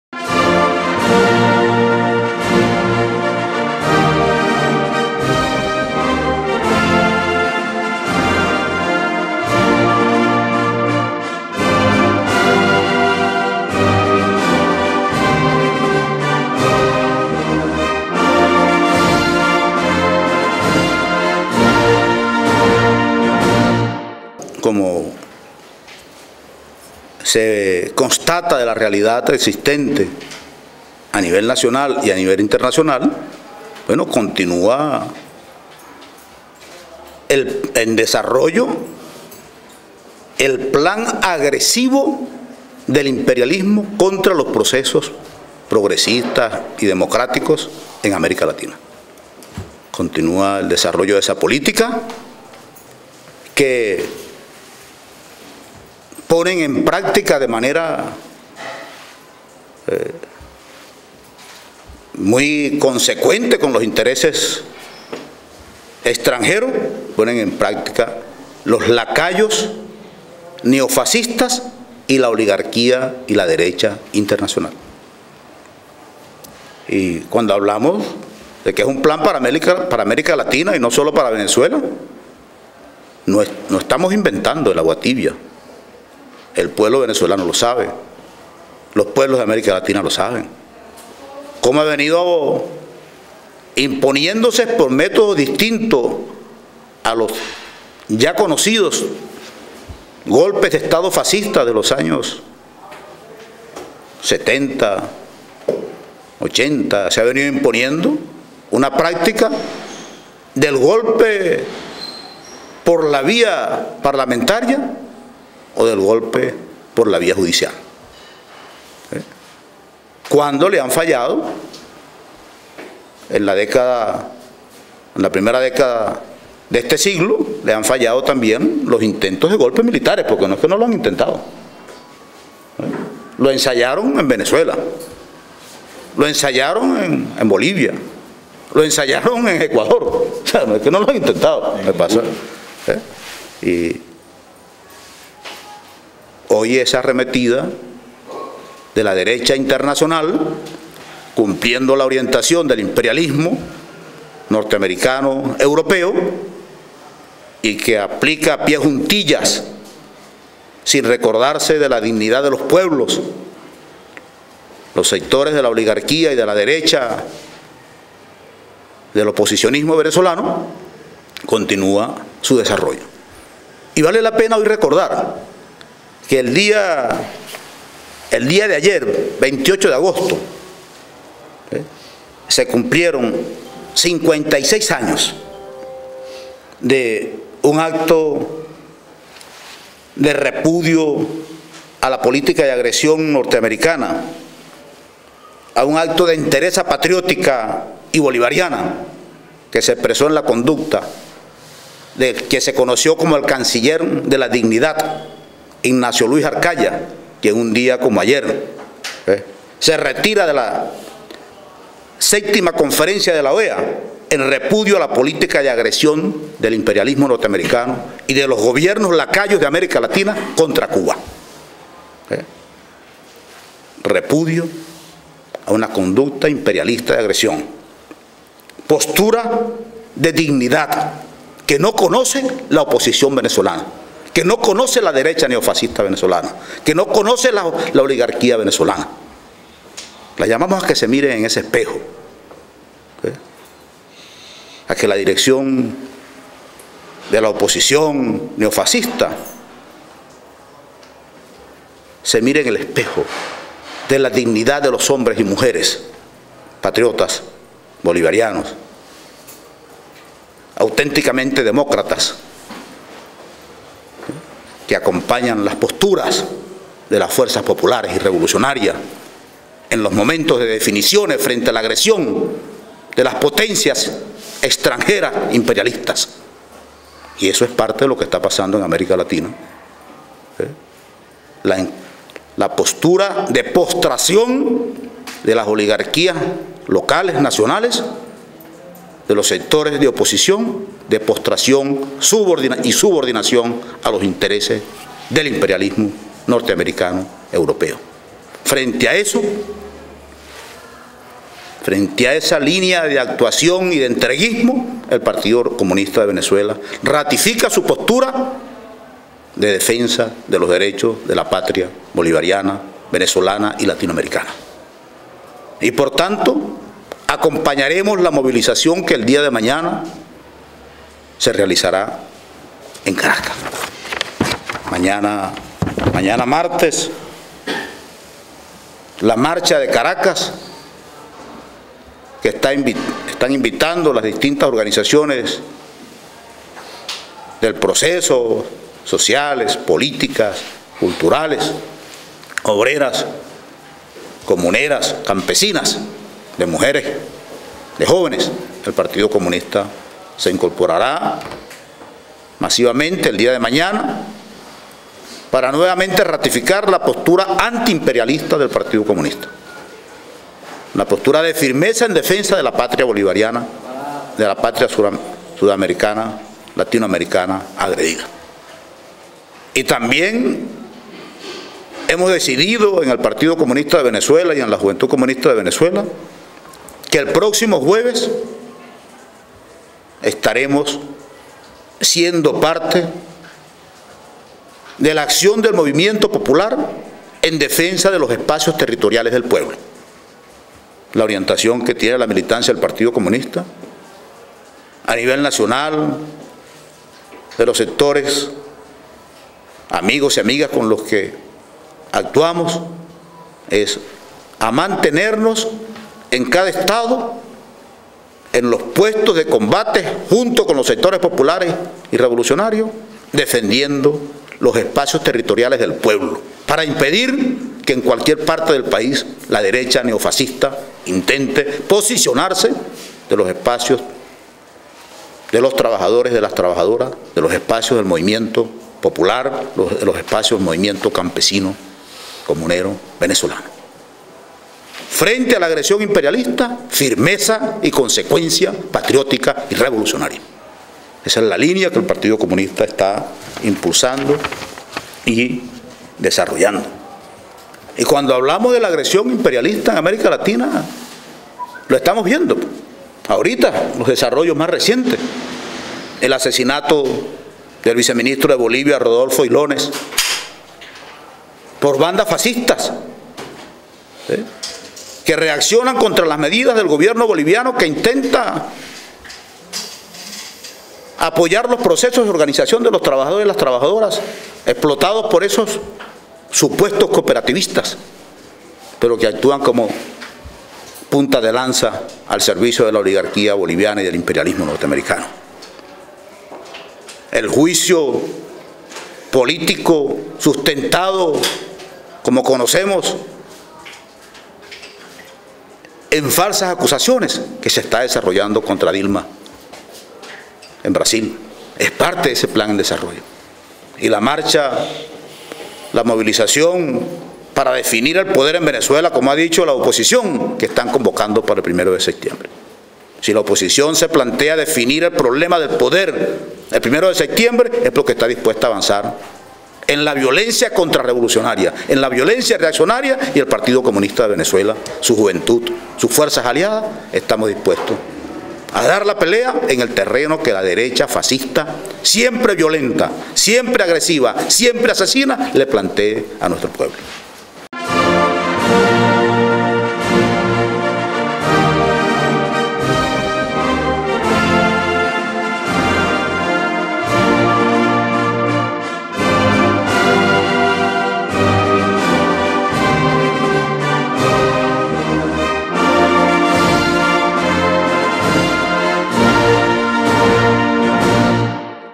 PCV llama a defender los espacios territoriales del pueblo ante agresión neofascista
Caracas, 29 ago. 2016, Tribuna Popular TP.- El Buró Político del Partido Comunista de Venezuela (PCV) reiteró la denuncia de agresión imperialista que se está desarrollando en América Latina y llamó al pueblo y a las fuerzas revolucionaria a defender los espacios territoriales ante los intentos violentos de la oposición.